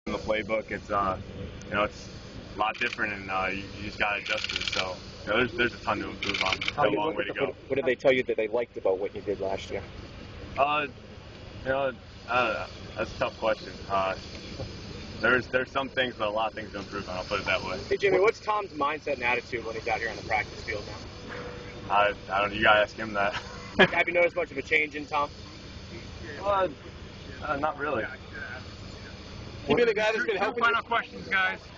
Patriots backup quarterbac Jimmy Garoppolo after Friday's OTA
Jimmy Garrapolo talks about his off-season workouts with the Patriots after this week;s third practice session.